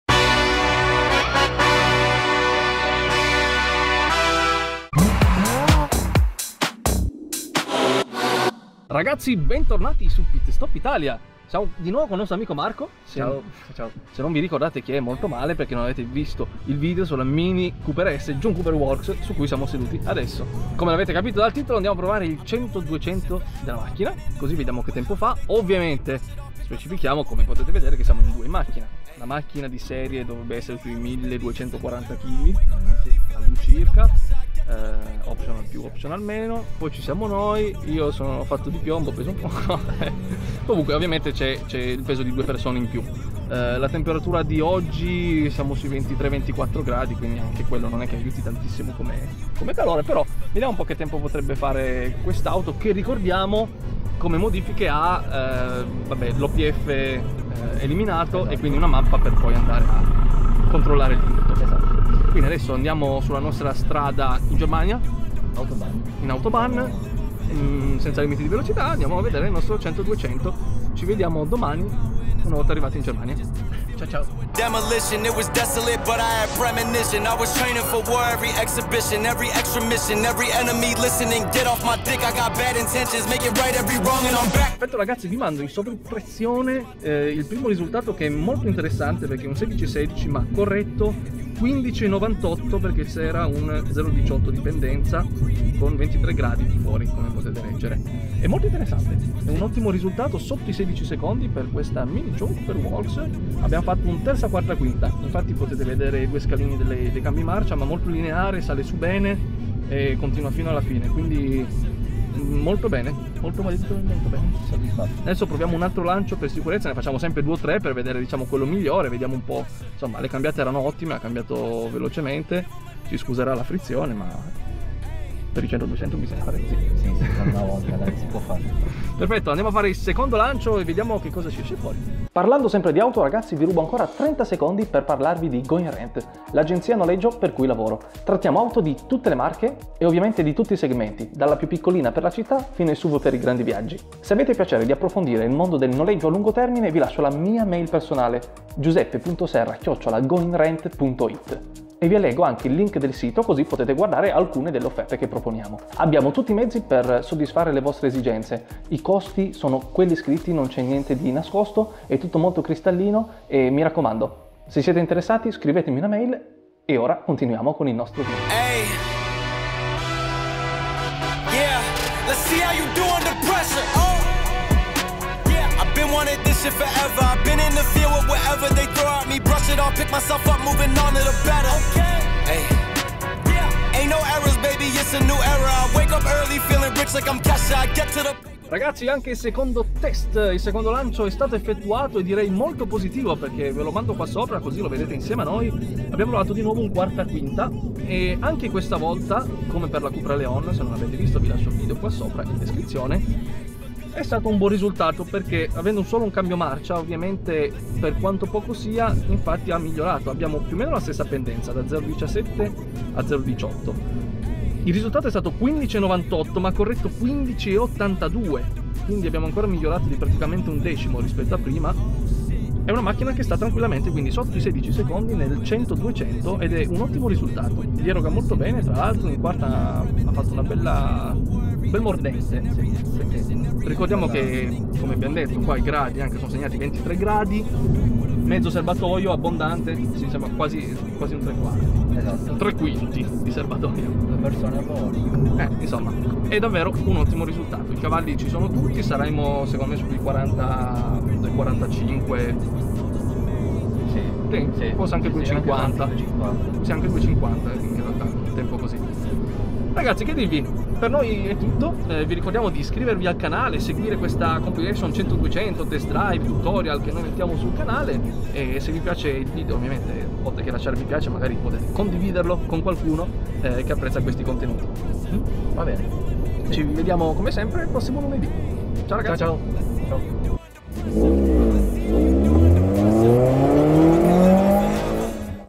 P-Gear 100 - 200 MINI Cooper S JCW | PSI
In questo nuovo test effettuato col P-Gear per calcolare il tempo sul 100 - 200 la protagonista è la Mini Cooper S John Cooper Works già testata precedentemente sul canale in un altro video:

La vettura ha l'eliminazione dell'OPF e una mappa soft.
I cavalli di serie sono 230 e vediamo con queste modifiche quanto ci vorrà per passare da 100 km/h a 200 km/h.